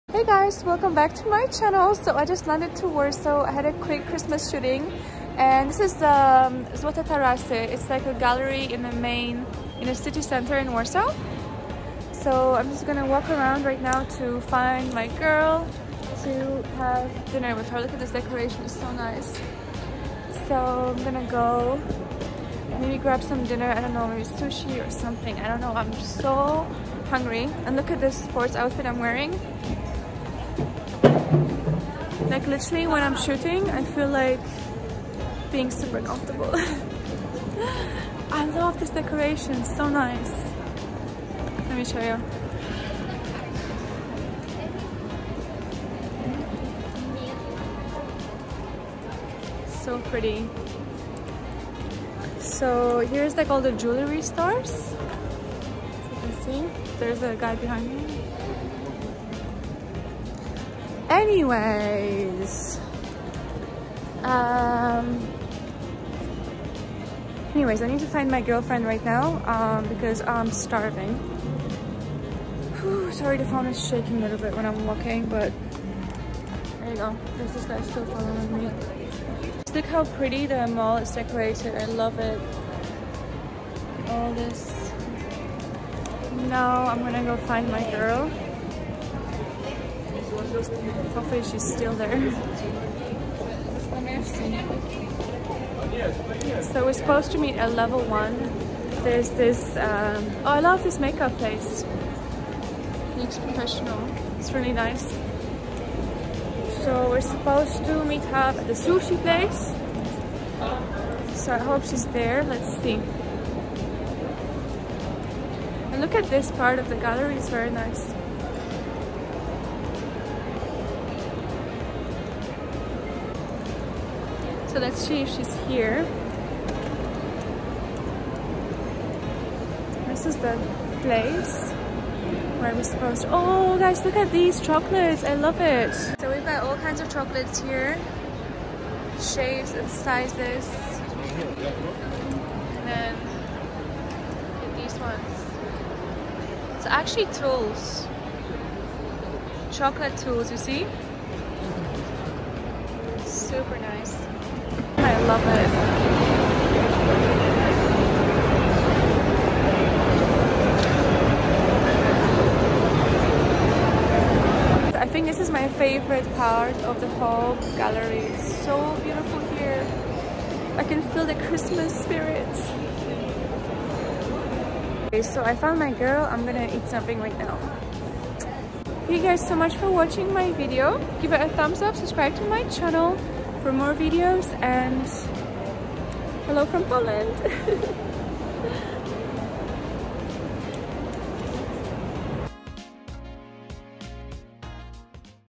Christmas Warsaw, Poland
In this episode of Anna Marisax show I would like to invite you to spend the whole day with me exploring Christmas in Warsaw - Poland.

Copy, browse or sharing the videos published on Anna Marisax youtube channel is not allowed. In cases of copy, publish or browse legal steps will be taken.